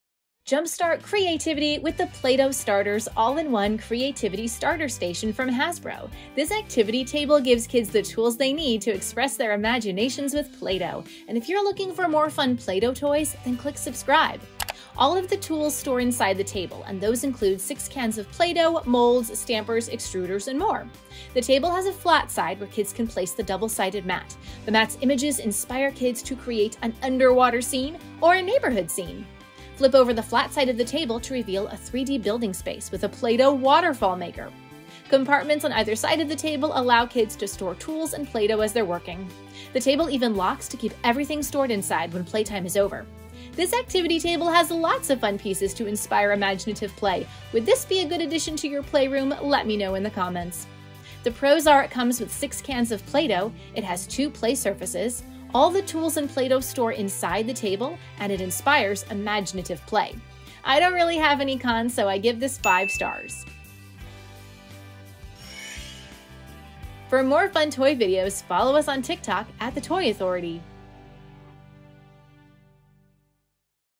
Play-Doh Starters All-in-One Creativity Starter Station
NEW Play-Doh Starters All-in-One Creativity Starter Station from Hasbro! Jumpstart creativity with this activity table! It comes with Play-Doh, tools, and two different play surfaces to inspire all sorts of Play-Doh creations. See how it works in this video review! For full review and shopping info

This gives Play-Doh fans everything they need to start creating! It's the Play-Doh Starters All-in-One Creativity Starter Station, and it's an activity table with storage, Play-Doh, tools, and two different play surfaces! It's got all you need to mold, build, and play with Play-Doh!